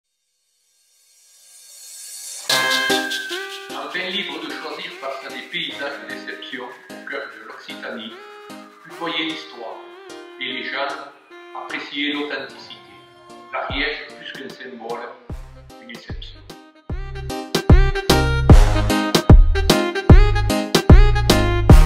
Gueules de nos Terroirs, Patrick Esteve, Ambassadeur de l'Ariège
Découvrez l'Ariège, avec Patrick Estève, Ambassadeur de son département de cœur